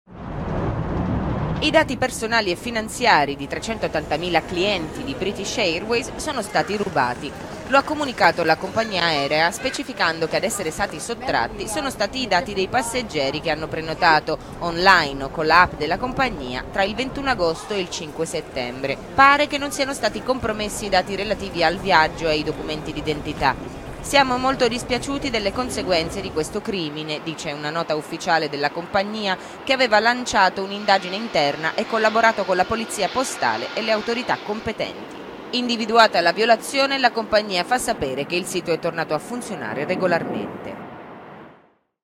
British Airways: rubati i dati di 380 mila passeggeri
La compagnia si scusa con i clienti dopo aver scoperto la violazione relativa a chi ha effettuato prenotazioni on line o sulla app.…